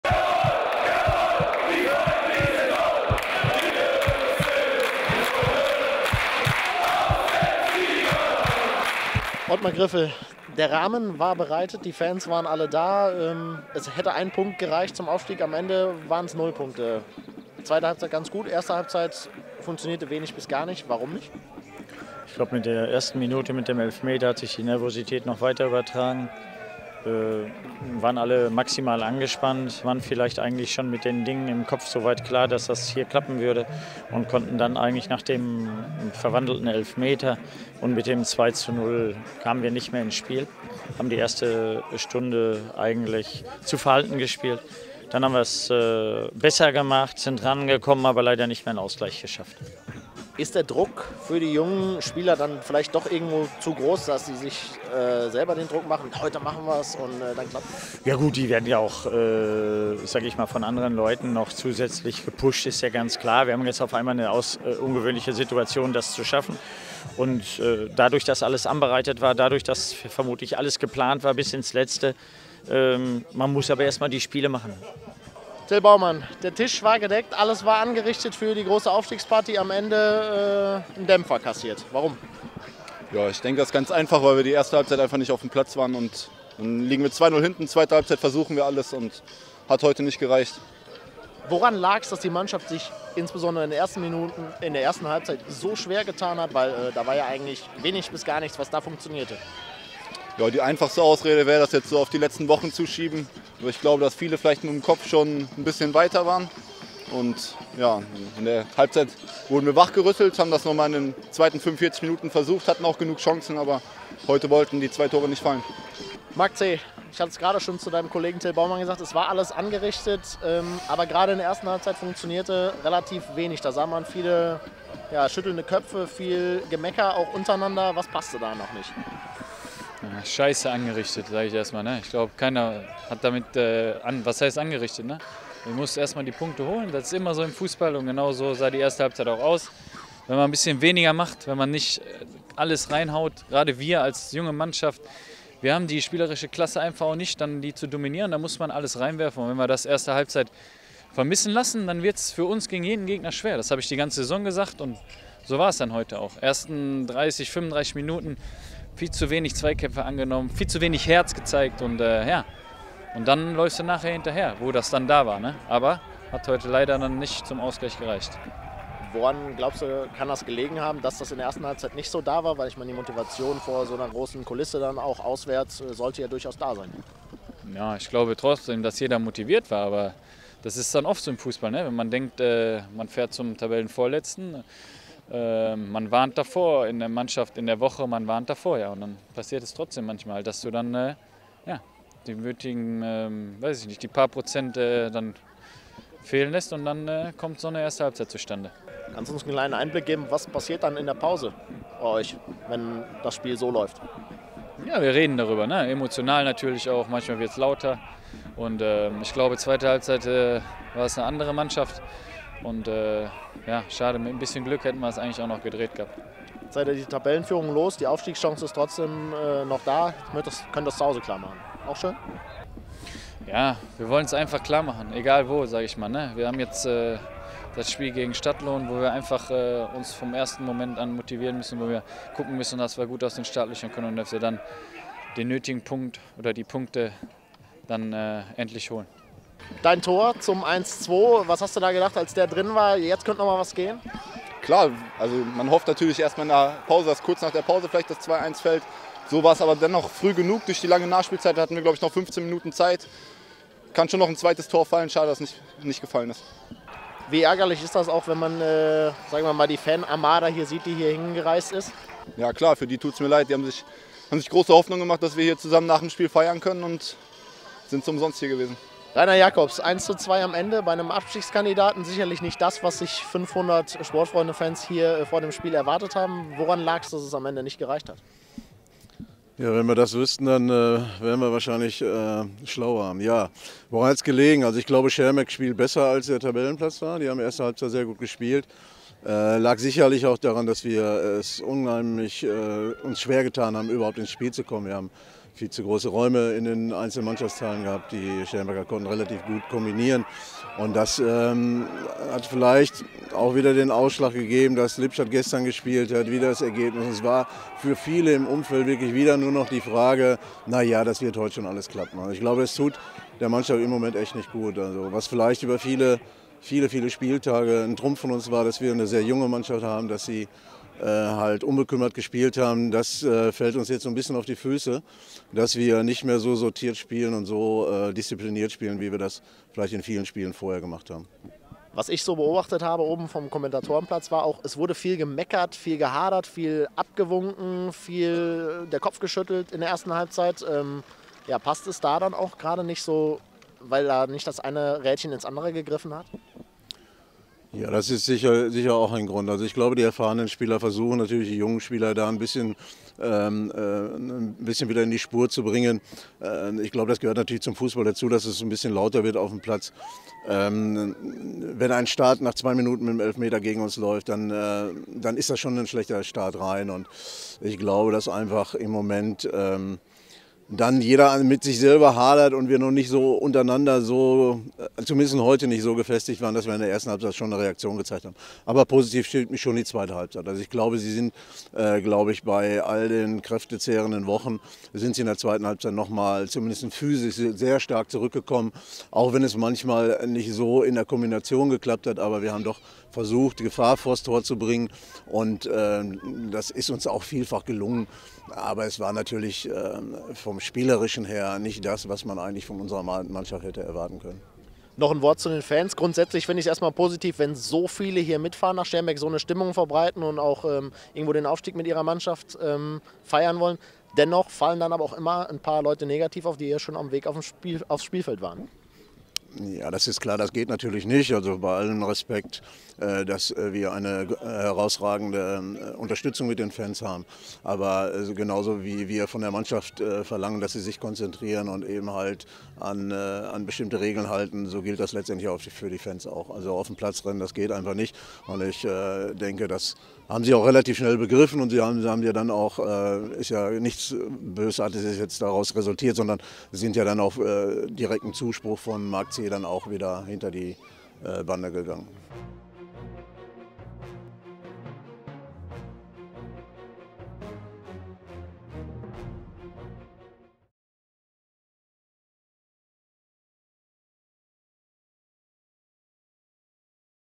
SV Schermbeck gegen Sportfreunde Siegen 2:1 (2:0)
SV Schermbeck gegen Sportfreunde Siegen 2:1 (2:0) - Im Interview Ottmar Griffel (Trainer Sportfreunde Siegen), Mark Zeh, Til Bauman und Reiner Jakobs (Vorstand Sport Sportfreunde Siegen)